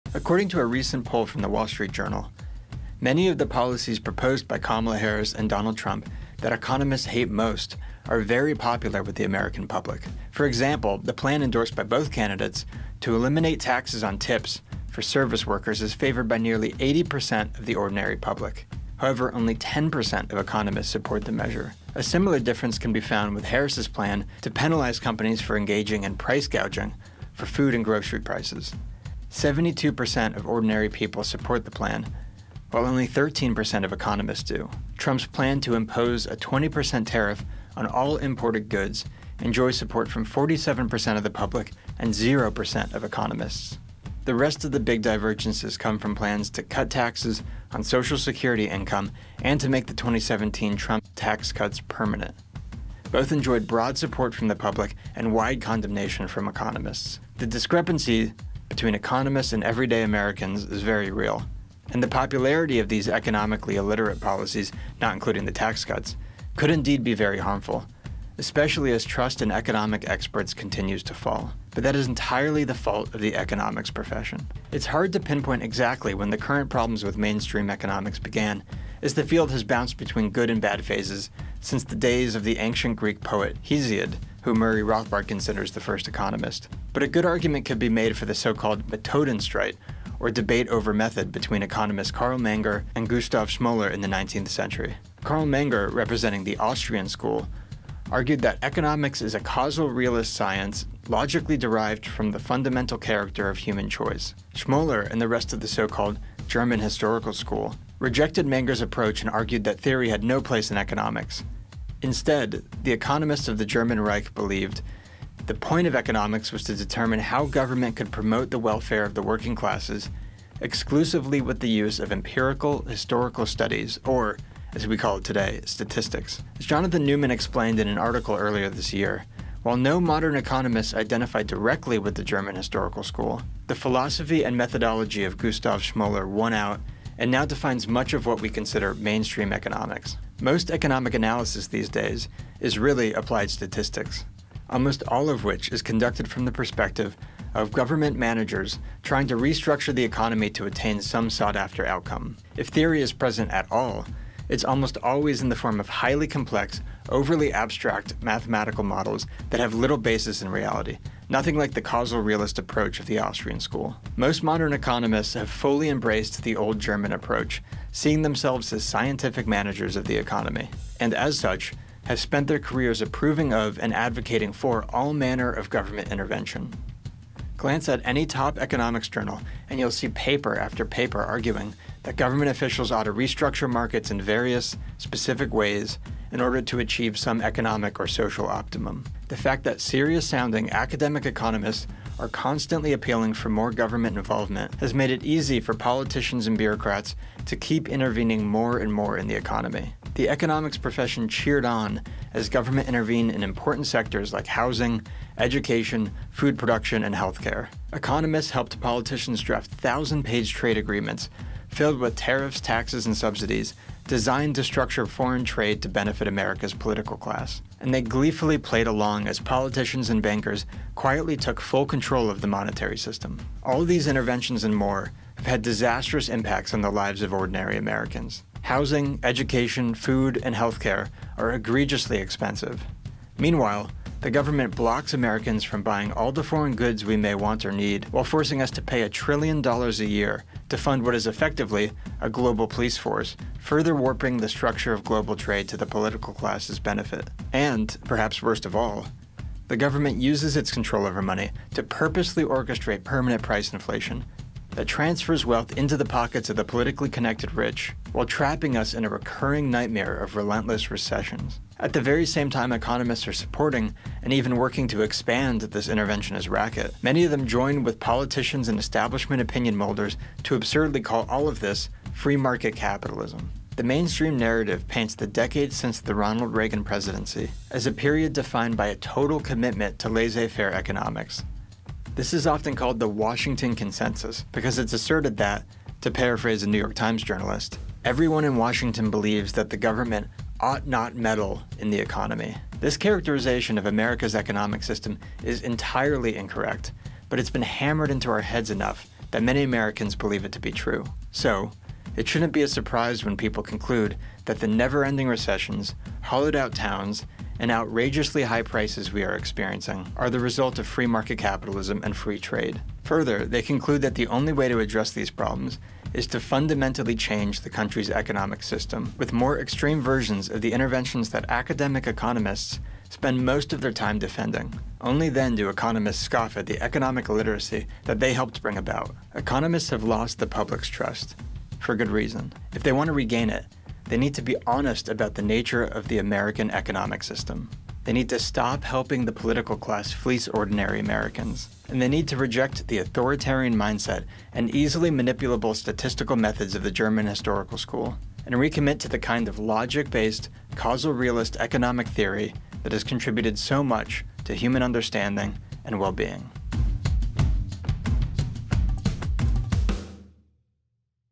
Economically-Illiterate Policy Proposals Are Popular, And Economists Are to Blame
Many “mainstream” economists are bothered by the popularity of economically-flawed policy proposals like tariffs and price controls. It’s their own fault.

Connor O’Keeffe produces media and content at the Mises Institute. He has a master’s in economics and a bachelor’s in geology.